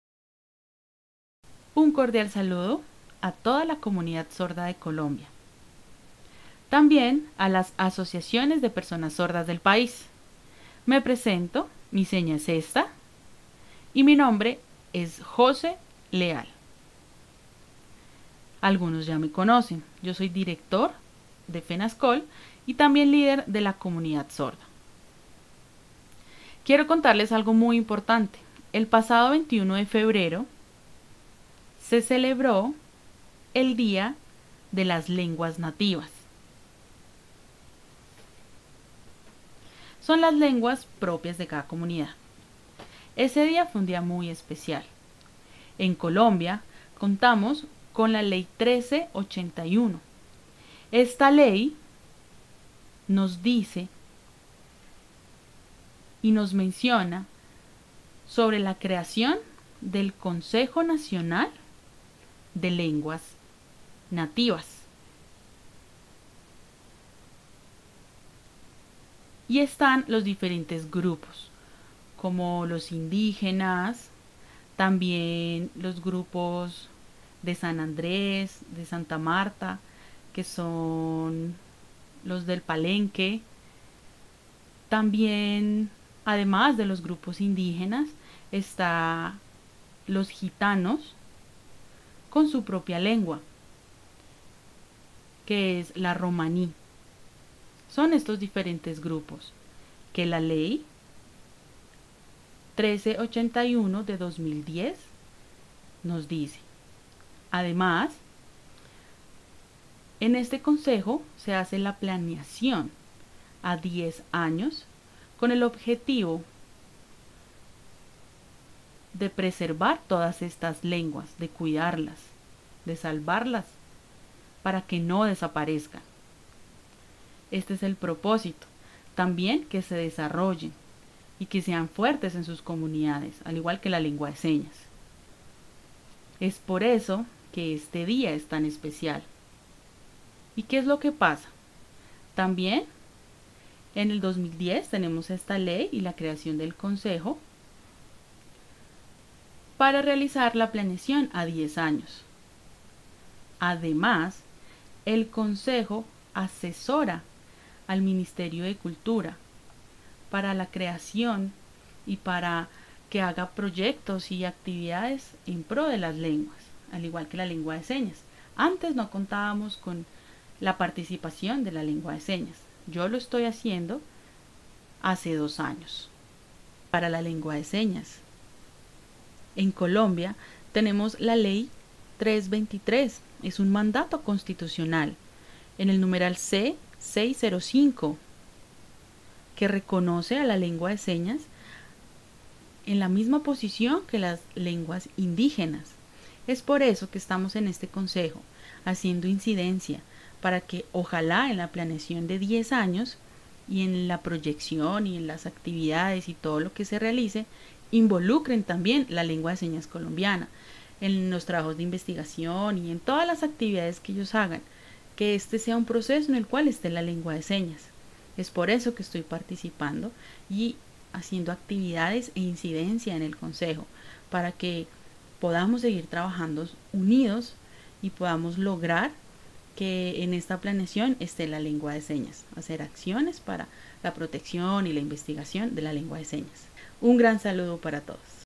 Fenascol informó sobre el lanzamiento del Plan Decenal de las Lenguas Nativas de Colombia
José Leal,director técnico de Fenascol informó sobre el lanzamiento del Plan Decenal de las Lenguas Nativas de Colombia en donde se incluye la Lengua de Señas Colombiana